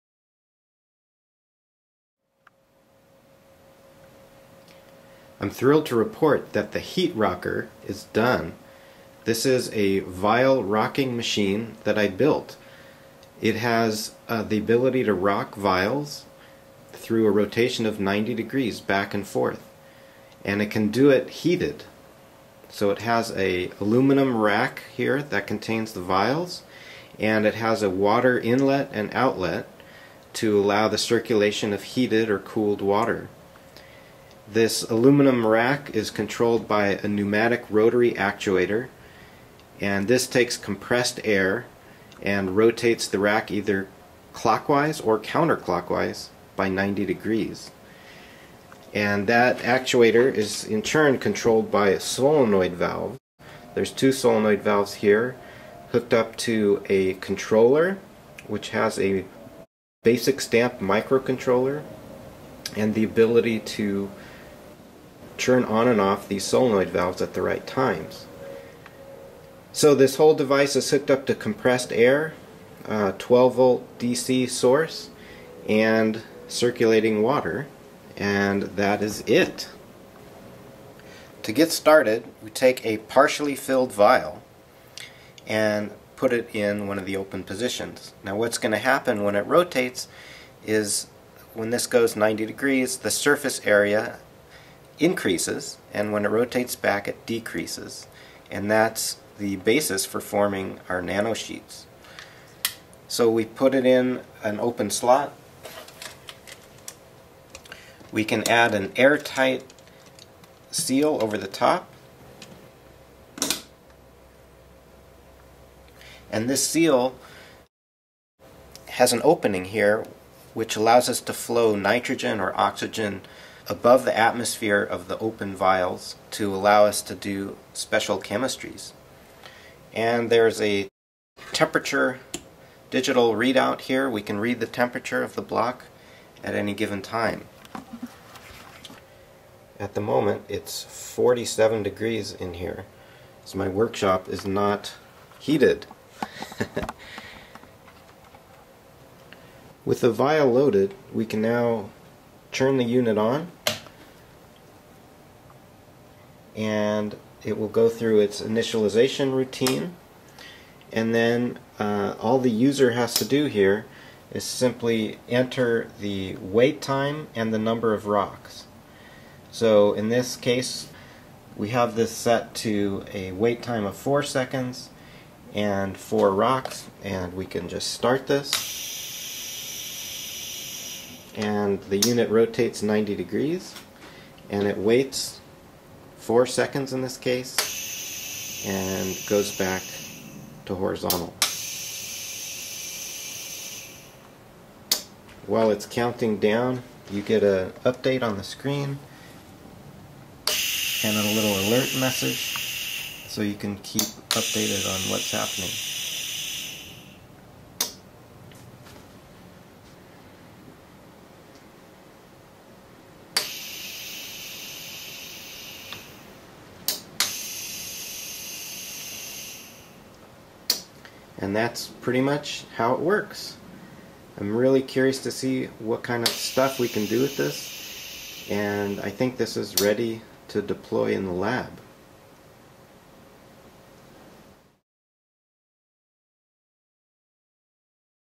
HeatRocker Instrument
This is an instrument I just completed that rotates vials back and forth by 90 degrees.  By controlling the wait time between rocks, and the total # of rocks we can control the self assembly of peptoid polymer solutions to make highly-ordered nanosheets.  This version of the device introduces temperature control, by means of an aluminum water-jacketed vial block.

For more on the science of nanosheet formation, you can check out our JACS paper:
"Shaken, not stirred: Collapsing a peptoid monolayer to produce free-floating, stable nanosheets". Sanii, B., et al., J. Am. Chem. Soc., 133, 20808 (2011).